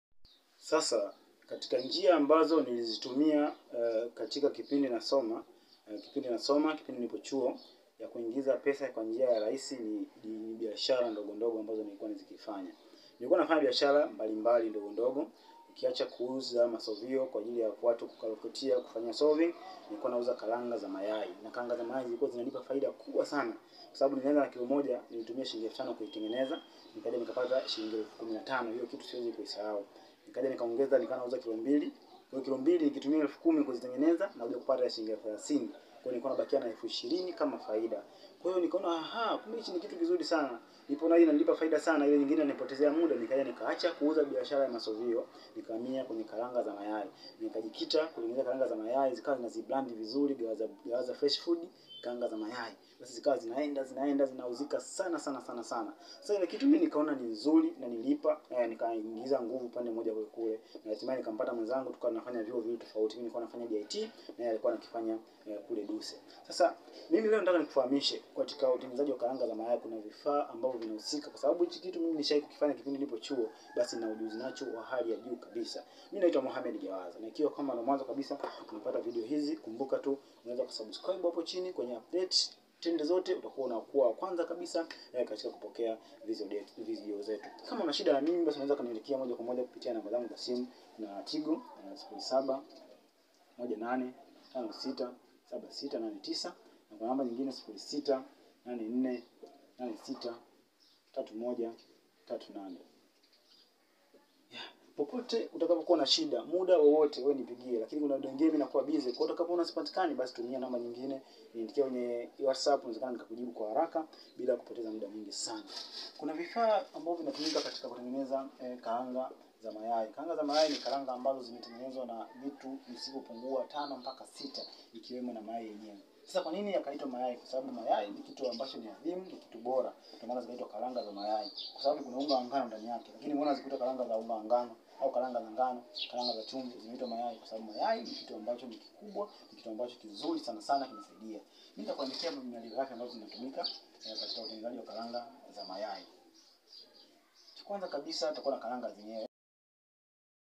TUJIFUNZE 1 : Mapishi ya KARANGA ZA MAYAI na FAIDA ZAKE
Leo katika UJASIRMALI twende pmoj kipi kinakupa faida zaidi, katika hiz biashara ya birtes wao waliofanikiwa wengi sana kupitia hii mradi wa karangaa z mayi, kama una biasha subiri nikwambie kitu.